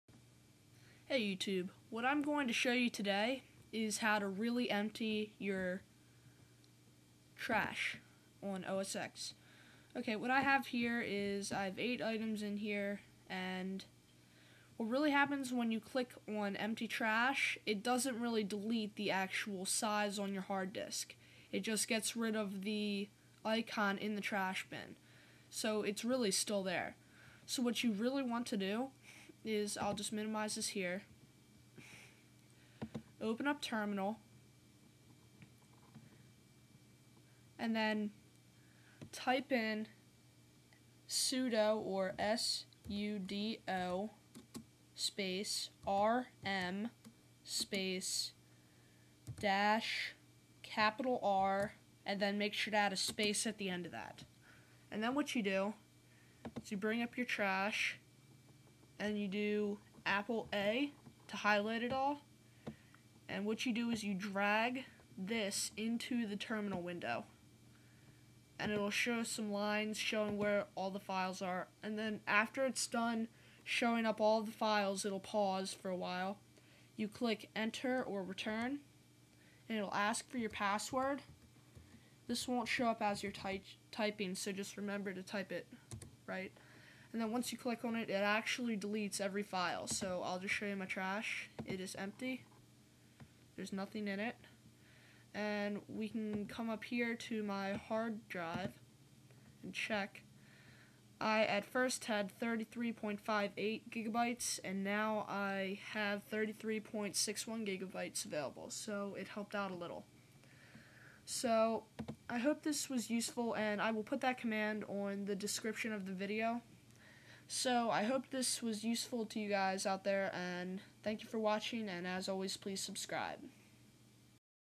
Really Empty Your Trash
This shows you how to really get rid of your trash.
Here is the command.
"sudo rm - R " dont copy the quotations.